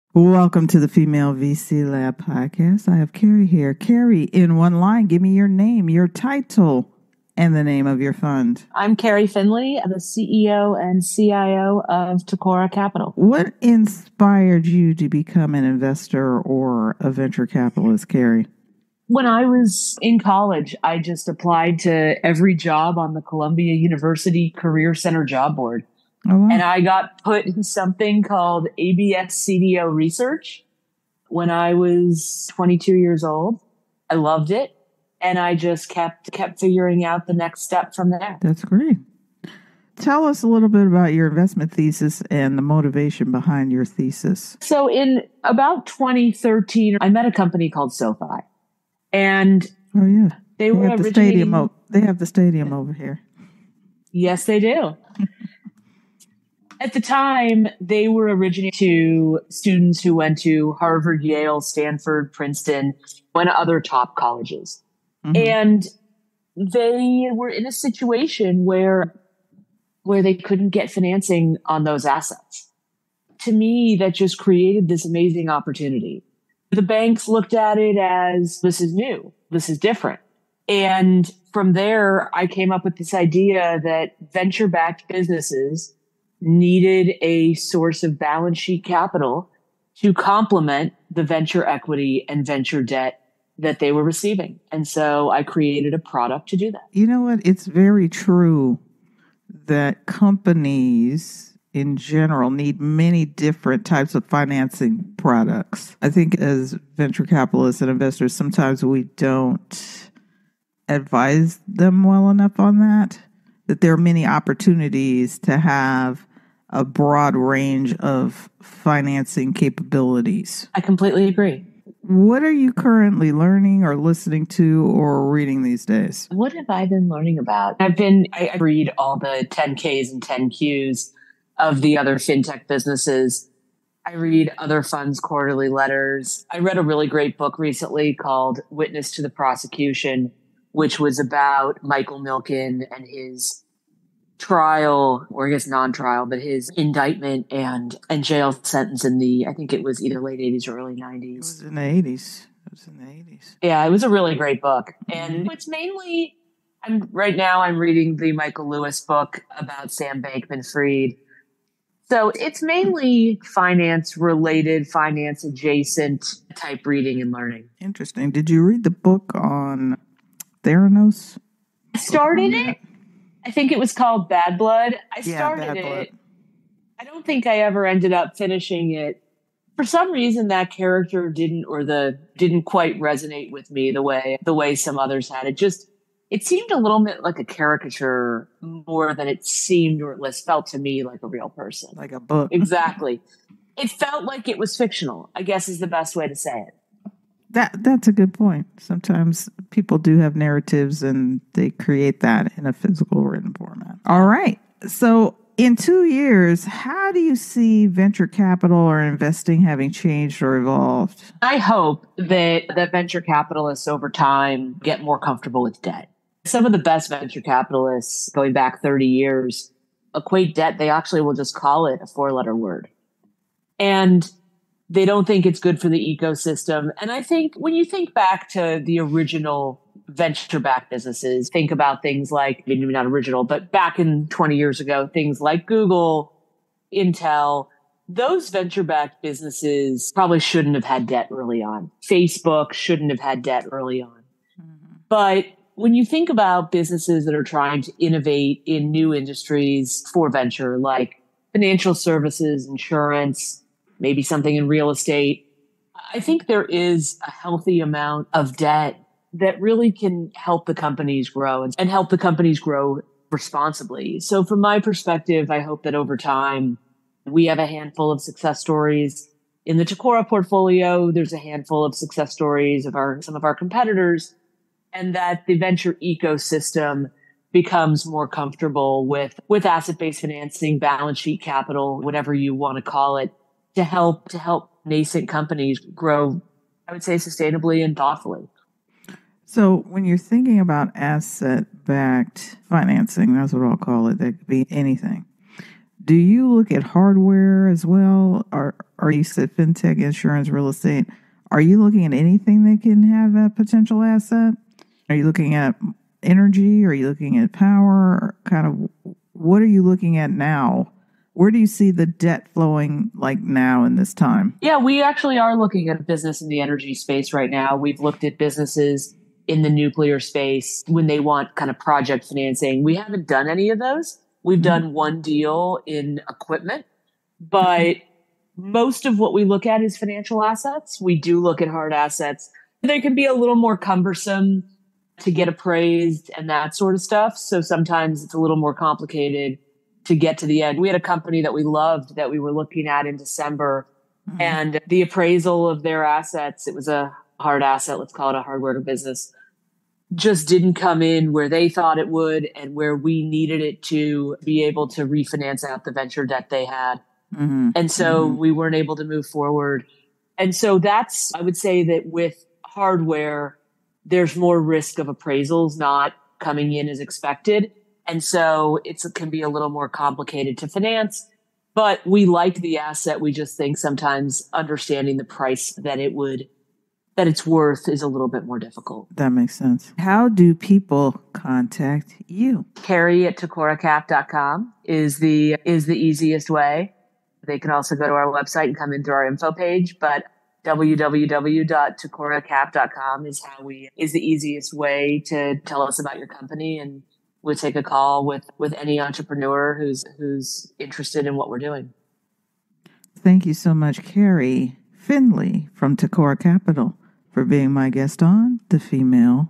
Female VC Lab - Episode 102: Keri Findley: The truth about Debt in Venture Capital 🚀
Welcome to another insightful episode of the Female VC Lab Podcast! In this episode, our host Barbara Bickham sits down with Keri Findley, the dynamic CEO and CIO of Tecora Capital.

Keri takes us through her intriguing journey from a college student applying to every job on the Columbia University Career Center job board to becoming a prominent figure in the venture capital world. She shares her unique investment thesis, inspired by her encounter with SoFi in 2013, and explains the importance of balance sheet capital for venture-backed businesses.

Listen in as Keri discusses the types of financing products that can broaden the growth potential for companies, her current readings and learnings in the finance realm, and her hopes for the future of venture capital—particularly around the integration and acceptance of debt in the ecosystem. She also gives us a sneak peek into the sectors Tecora Capital is currently exploring, including energy and hard assets.

📚 **Books Mentioned:**

- Witness to the Prosecution (about Michael Milken)

- Michael Lewis's book on Sam Bankman Fried

- Bad Blood by John Carreyrou

CHAPTERS:
00:00 - Welcome
00:15 - Inspiration to Become an Investor
00:40 - Investment Thesis Explained
02:02 - Current Learning Journey
03:24 - Future of Investing in 2 Years
06:19 - Contact Information